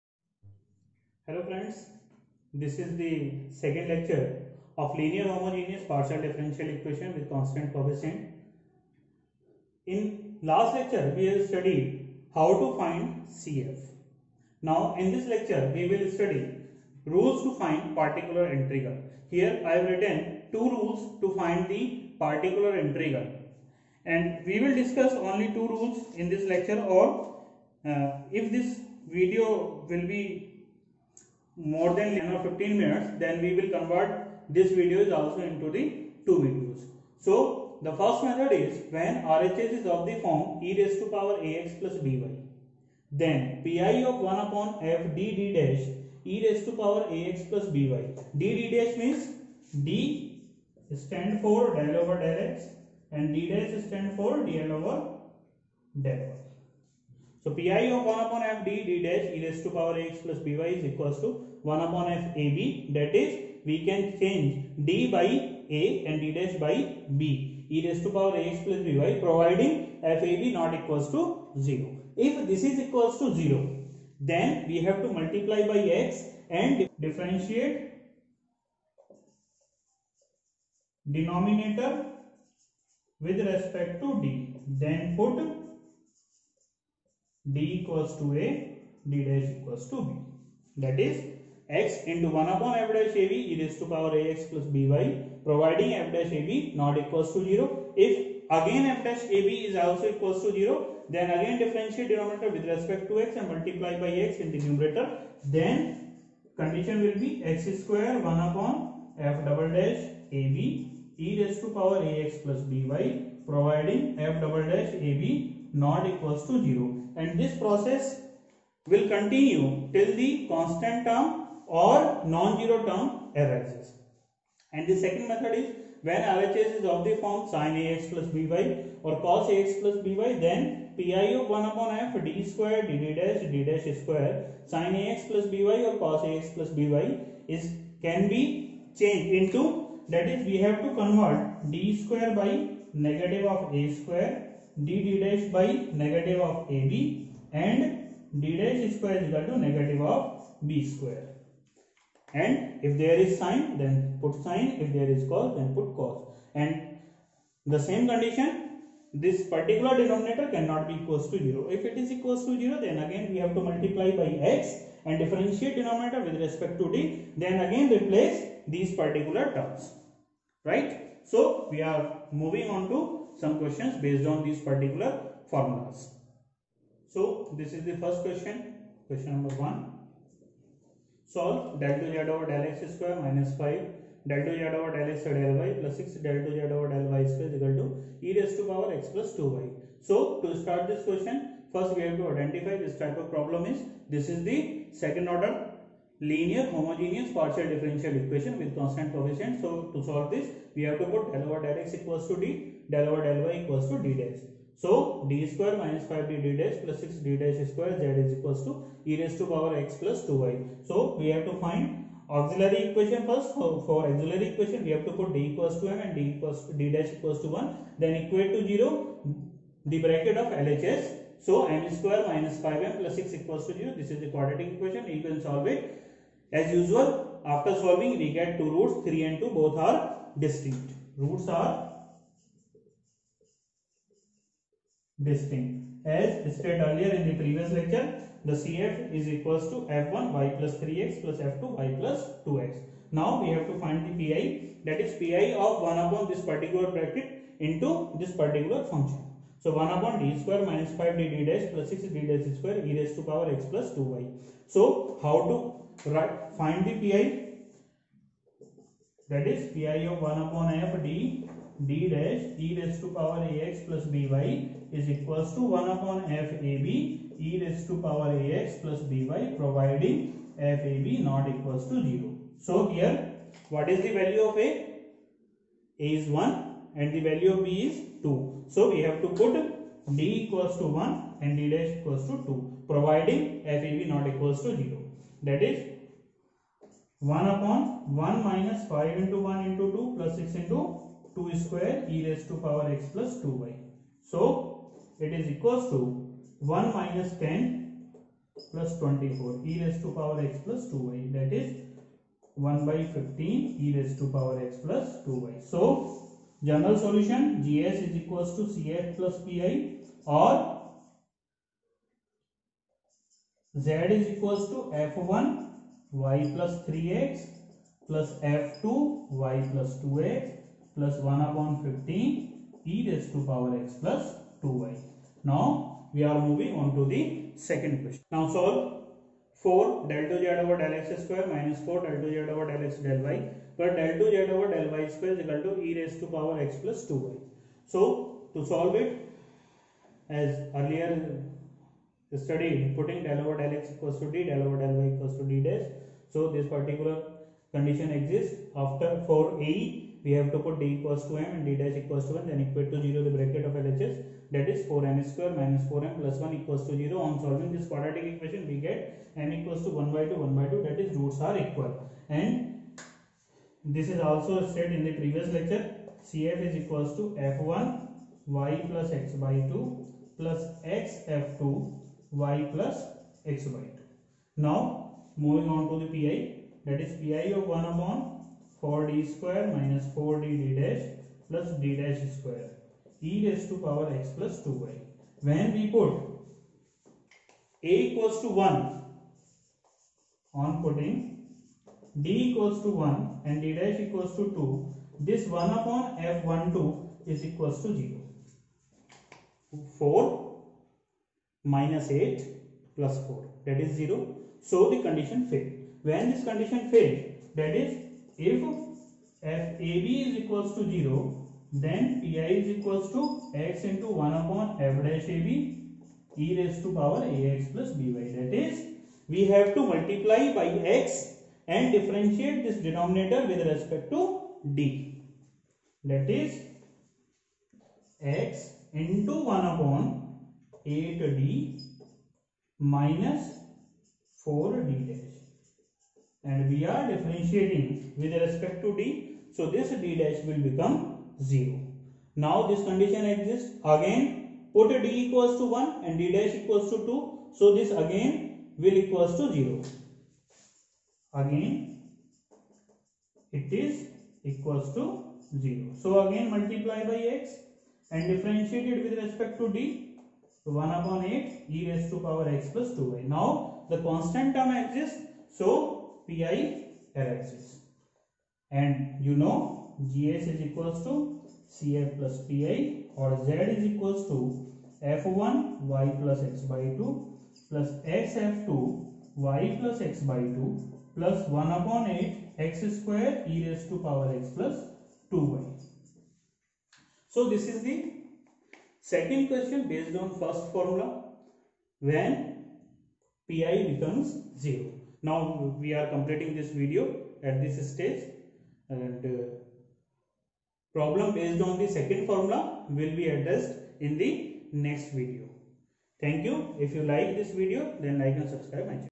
Homogeneous Partial Differential Equation with Constant Coefficient: Particular Integral:Lecture-2.1
An equation containing one or more partial derivatives of an un known function of two or more independent variables is known as a (P.D.E.).
Order of a Partial Differential Equation (O.P.D.E.): The order of a partial differential equation is defined as the order of the highest partial derivative occurring in the partial differential equation.
Degree of a Partial Differential Equation (D.P.D.E.):The degree of a partial differential equation is the degree of the highest order derivative which occurs in it after the equation has been rationalized, i.e made free from radicals and fractions so for as derivatives are concerned.